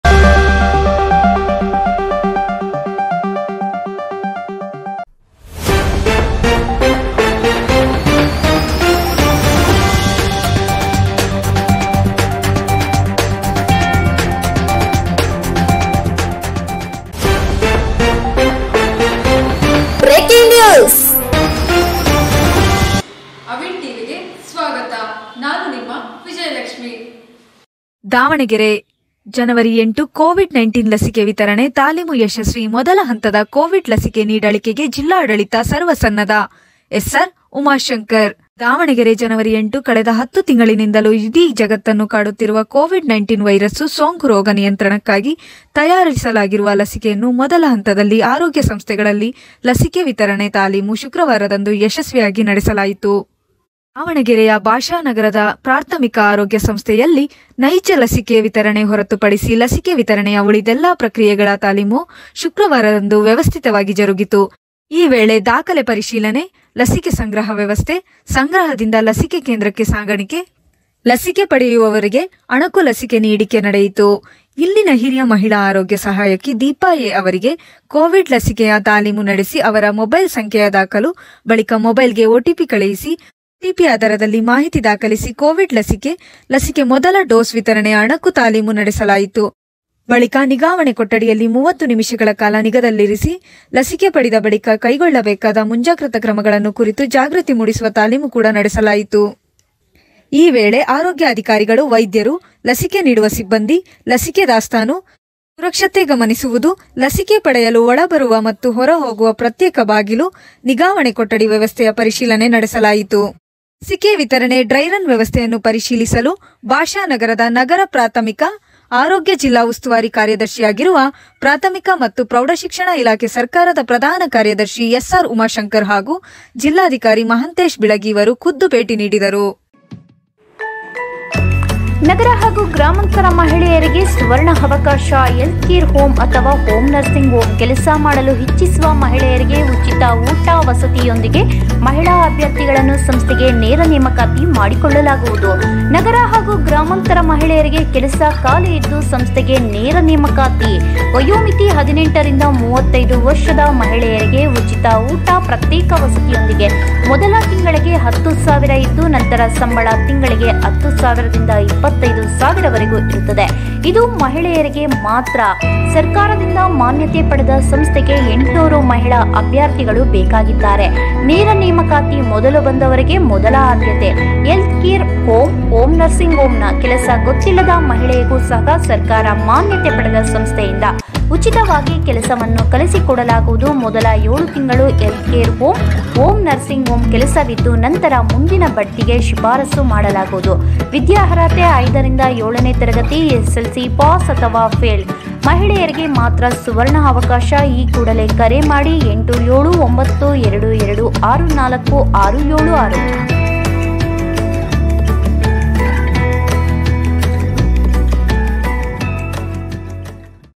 ಮೊದಲ ಹಂತದ ಕೋವಿಡ್ ಲಸಿಕೆ ನೀಡಿಕೆಗೆ ಜಿಲ್ಲಾಡಳಿತ ಸರ್ವ ಸನ್ನದ್ಧ- ಎಸ್.ಆರ್. ಉಮಾಶಂಕರ್
ಟಿವಿ ಸುದ್ದಿ ವಾಹಿನಿಯಲ್ಲಿ ಕಾರ್ಯನಿರ್ವಹಿಸಲು ಜಿಲ್ಲಾ/ ತಾಲುಕಾ ಅನುಭವಿ ವರದಿಗಾರರ ಬೇಕಾಗಿದ್ದಾರೆ

ಸಂಪರ್ಕಿಸಿ

ಕೋವಿಡ್ 19 ಲಸಿಕೆ ವಿತರಣೆ ತಾಲೀಮು ಯಶಸ್ವಿ
ದಾವಣಗೆರೆ ಜ. 08 (ಕರ್ನಾಟಕ ವಾರ್ತೆ):
ಕಳೆದ  10 ತಿಂಗಳಿನಿಂದಲೂ ಇಡೀ ಜಗತ್ತನ್ನು ಕಾಡುತ್ತಿರುವ ಕೋವಿಡ್-19 ವೈರಸ್ ಸೋಂಕು ರೋಗ  ನಿಯಂತ್ರಣಕ್ಕಾಗಿ ತಯಾರಿಸಲಾಗಿರುವ ಲಸಿಕೆಯನ್ನು ಮೊದಲ ಹಂತದಲ್ಲಿ ಆರೋಗ್ಯ  ಕಾರ್ಯಕರ್ತರಿಗೆ ನೀಡಲು ಯೋಜಿಸಿದ್ದು, ಸರ್ಕಾರದ ನಿರ್ದೇಶನದಂತೆ ಇದಕ್ಕಾಗಿ ಜಿಲ್ಲೆಯ 06  ಆರೋಗ್ಯ ಸಂಸ್ಥೆಗಳಲ್ಲಿ ಲಸಿಕೆ ವಿತರಣೆ ತಾಲೀಮು ಶುಕ್ರವಾರದಂದು ಯಶಸ್ವಿಯಾಗಿ  ನಡೆಸಲಾಯಿತು.
    ದಾವಣಗೆರೆಯ ಬಾಷಾ ನಗರದ ನಗರ ಪ್ರಾಥಮಿಕ ಆರೋಗ್ಯ ಸಂಸ್ಥೆಯಲ್ಲಿ  ನೈಜ ಲಸಿಕೆ ವಿತರಣೆ ಹೊರತುಪಡಿಸಿ, ಲಸಿಕೆ ವಿತರಣೆಯ ಉಳಿದೆಲ್ಲ ಪ್ರಕ್ರಿಯೆಗಳ ತಾಲೀಮು  ಶುಕ್ರವಾರದಂದು ವ್ಯವಸ್ಥಿತವಾಗಿ ಜರುಗಿತು.  ಈ ವೇಳೆ ದಾಖಲೆ ಪರಿಶೀಲನೆ, ಲಸಿಕೆ ಸಂಗ್ರಹ  ವ್ಯವಸ್ಥೆ, ಸಂಗ್ರಹಗಾರದಿಂದ ಲಸಿಕೆ ಕೇಂದ್ರಕ್ಕೆ ಸಾಗಾಣಿಕೆ, ಲಸಿಕೆ ಪಡೆಯುವವರಿಗೆ  ಅಣುಕು ಲಸಿಕೆ ನೀಡಿಕೆ ನಡೆಯಿತು.  ಇಲ್ಲಿನ ಹಿರಿಯ ಮಹಿಳಾ ಆರೋಗ್ಯ ಸಹಾಯಕಿ ದೀಪಾ ಎ  ಅವರಿಗೆ ಕೋವಿಡ್ ಲಸಿಕೆಯ ತಾಲೀಮು ನಡೆಸಿ, ಅವರ ಮೊಬೈಲ್ ಸಂಖ್ಯೆಯ ದಾಖಲು, ಬಳಿಕ  ಮೊಬೈಲ್ಗೆ ಒಟಿಪಿ ಕಳುಹಿಸಿ, ಒಟಿಪಿ ಆಧಾರದಲ್ಲಿ ಮಾಹಿತಿ ದಾಖಲಿಸಿ, ಕೋವಿಡ್ ಲಸಿಕೆ  ಮೊದಲ ಡೋಸ್ ವಿತರಣೆಯ ಅಣುಕು ತಾಲೀಮು ನಡೆಸಲಾಯಿತು.  ಬಳಿಕ ನಿಗಾವಣೆ ಕೊಠಡಿಯಲ್ಲಿ 30  ನಿಮಿಷಗಳ ಕಾಲ ನಿಗಾದಲ್ಲಿರಿಸಿ, ಲಸಿಕೆ ಪಡೆದ ಬಳಿಕ ಕೈಗೊಳ್ಳಬೇಕಾದ ಮುಂಜಾಗ್ರತಾ  ಕ್ರಮಗಳ ಕುರಿತ ಕರಪತ್ರ ನೀಡಿಕೆ.  28 ದಿನಗಳ ಬಳಿಕ ಎರಡನೆÉ ಡೋಸ್ ಪಡೆಯಲು  ಅನುಸರಿಸಬೇಕಾದ ಕ್ರಮಗಳ ಕುರಿತು ಜಾಗೃತಿ ಮೂಡಿಸುವ ತಾಲೀಮು ಕೂಡ ನಡೆಸಲಾಯಿತು.  ಈ ವೇಳೆ  ಆರೋಗ್ಯ ಅಧಿಕಾರಿಗಳು, ವೈದ್ಯರು, ಲಸಿಕೆ ನೀಡುವ ಸಿಬ್ಬಂದಿ, ಶುಶ್ರೂಷಕರು, ಸಹಾಯಕ  ಸಿಬ್ಬಂದಿ, ಭದ್ರತಾ ಸಿಬ್ಬಂದಿ, ಲಸಿಕೆ ದಾಸ್ತಾನು, ಸುರಕ್ಷತೆ ಗಮನಿಸುವುದು,  ಲಸಿಕೆ  ಪಡೆಯಲು ಒಳ ಬರಲು ಮತ್ತು ಹೊರ ಹೋಗಲು ಪ್ರತ್ಯೇಕ ಬಾಗಿಲು, ನಿಗಾವಣೆ ಕೊಠಡಿ ವ್ಯವಸ್ಥೆಯ  ಪರಿಶೀಲನೆ ನಡೆಸಲಾಯಿತು.
    ಲಸಿಕೆ ವಿತರಣೆ ಡ್ರೈರನ್ ವ್ಯವಸ್ಥೆಯನ್ನು  ಪರಿಶೀಲಿಸಲು ಬಾಷಾ ನಗರದ ನಗರ ಪ್ರಾಥಮಿಕ ಆರೋಗ್ಯಕ್ಕೆ ಜಿಲ್ಲಾ ಉಸ್ತುವಾರಿ  ಕಾರ್ಯದರ್ಶಿಯಾಗಿರುವ ಪ್ರಾಥಮಿಕ ಮತ್ತು ಪ್ರೌಢಶಿಕ್ಷಣ ಇಲಾಖೆ ಸರ್ಕಾರದ ಪ್ರಧಾನ  ಕಾರ್ಯದರ್ಶಿ ಎಸ್.ಆರ್. ಉಮಾಶಂಕರ್, ಜಿಲ್ಲಾಧಿಕಾರಿ ಮಹಾಂತೇಶ್ ಬೀಳಗಿ ಅವರು ಖುದ್ದು  ಭೇಟಿ ನೀಡಿದರು.